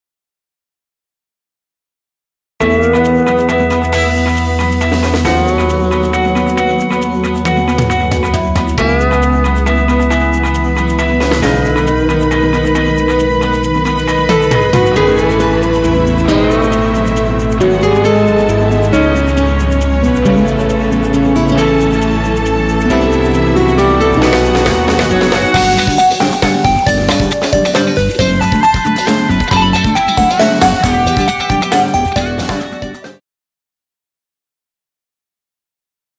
HAKKO HU-100; 4 Axis Table Top Type Soldering Robot 2018
Anyone can create programs with simple input HAKKO original 4 axis table-top type auto-soldering robot system.